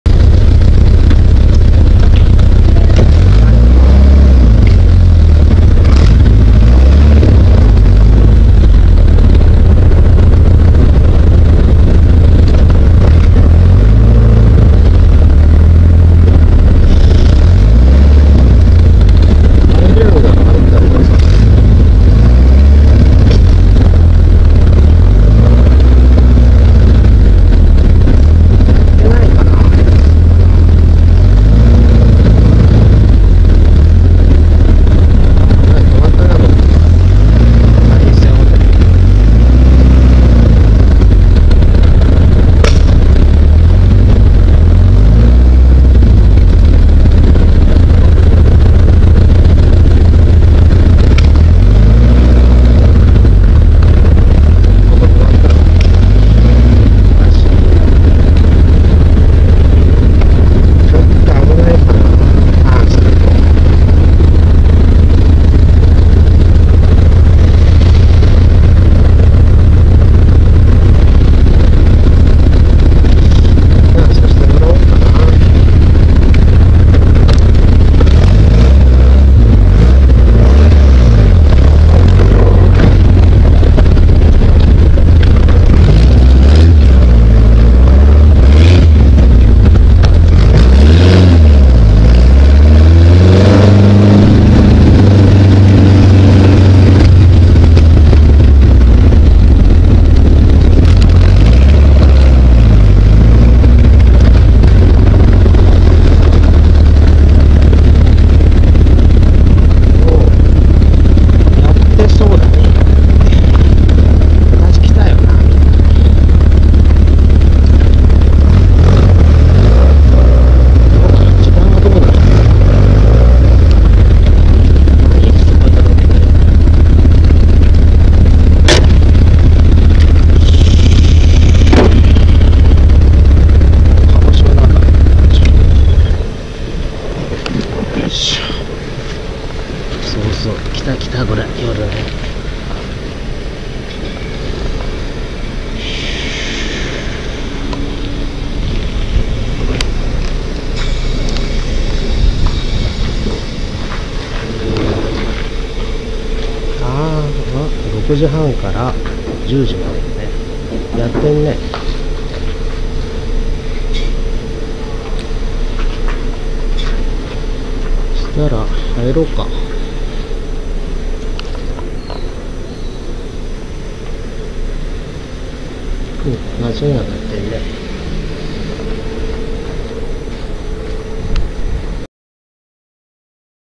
121008湯布院「乙丸温泉」。
ブログ用。湯布院駅近く、の温泉です。仲間内と15年ほど前に来た記憶が・・・。